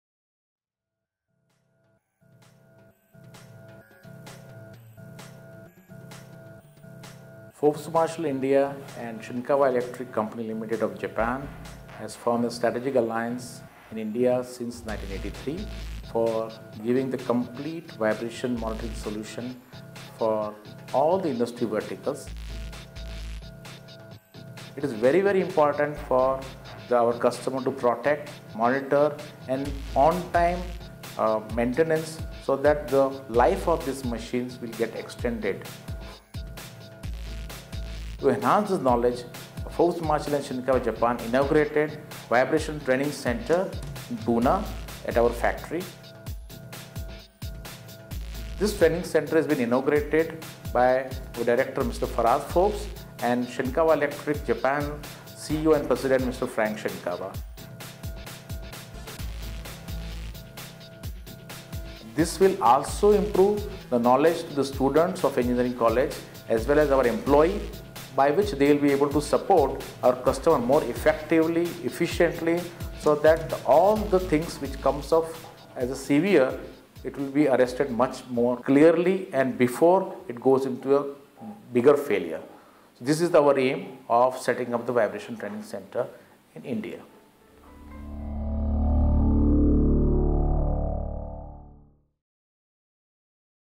Forbes Marshall Vibration Monitoring Training Centre
A Vibration Monitoring Training Center was inaugurated at our Pune, MIDC premises on August 26th, 2014. Mr. Frank Shinkawa, president & CEO of Shinkawa Electric Company Limited, Japan and Mr. Farhad Forbes, Director Forbes Marshall, cut the ribbon on this occasion. Mr. Mukesh Vyas, BU Head, Vibration Monitoring Division, explained about our plans for this training center. Mr. Farhad Forbes, Mr. Kekoo Vacha, Mr. Jehangir Ardeshir also shared their views on the same.

About the training center :
This training center is mainly for giving training to our customers, end users and our own members on topics like basics of vibration, how can one proactively maintain rotating machines with the help of vibration monitoring & analysis etc. We also aim at starting Vibration Courses for customers  on a later stage.